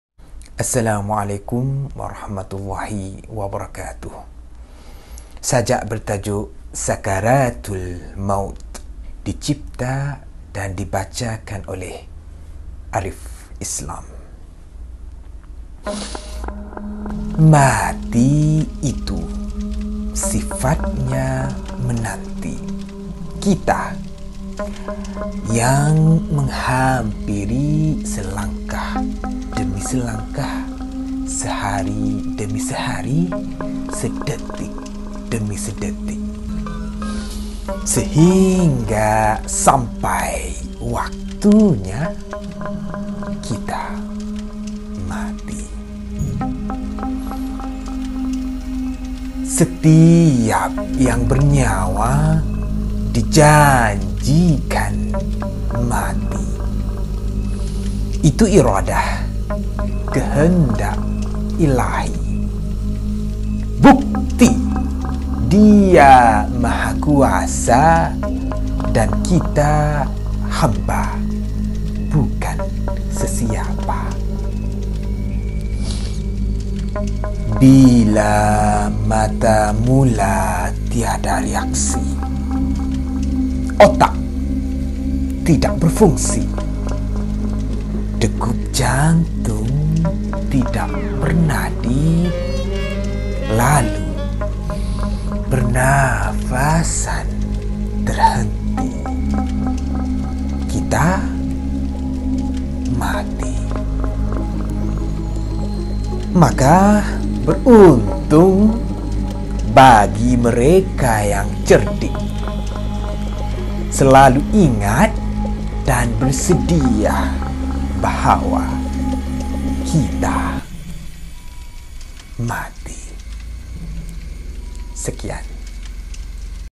Sajak : SAKARATULMAUT
Assalamualaikum wrt.

Nukilan dan deklamasi oleh ambe sendiri.